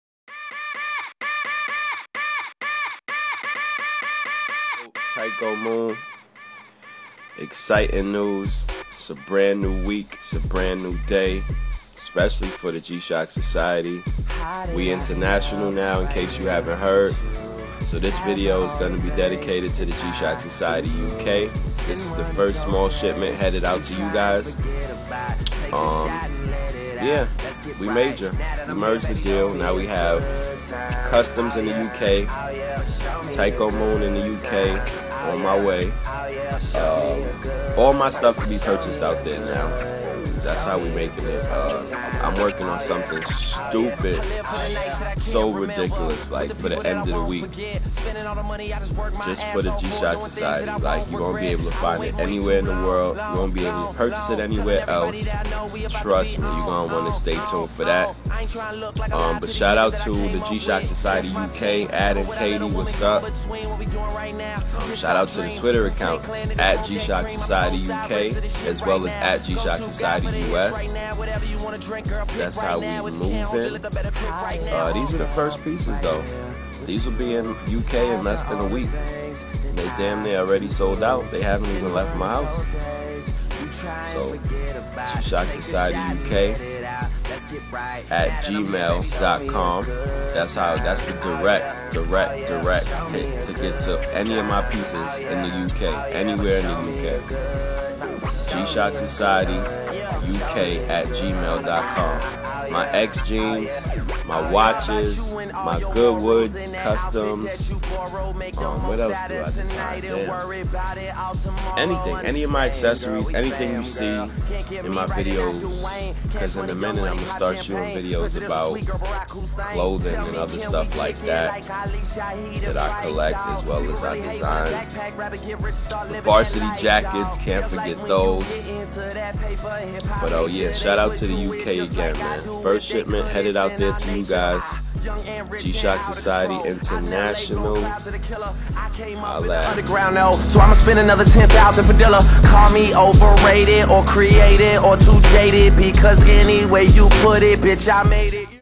G- shock shipment to The UK!!!! Gshock Society UK making history
So if u haven't heard Tyko has branched out his company to the United Kingdom! And here are some of the pieces being shipped out to the GshockSocietyUK!!!